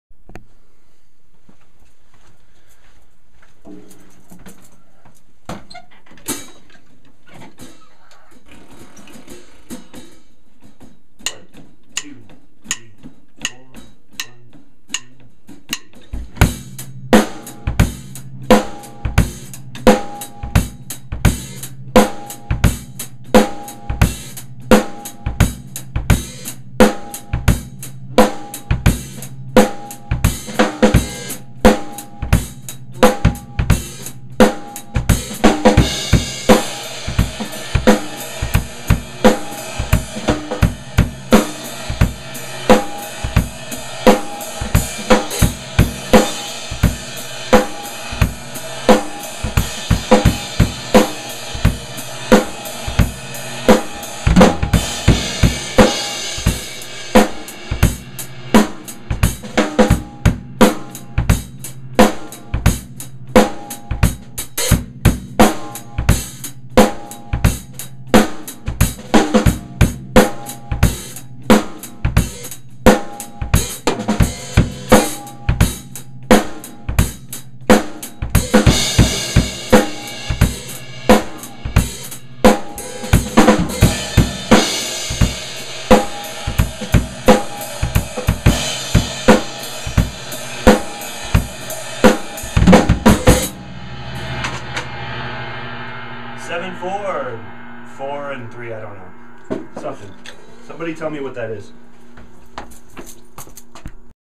Drum Beats: Odd time beats, is it 7/4 or 4/4 and 3/4? i hate fractions
Smart drummers needed! someone tell me what time signature this is, i just know i count 4 then 3. you guys are the best, educate me!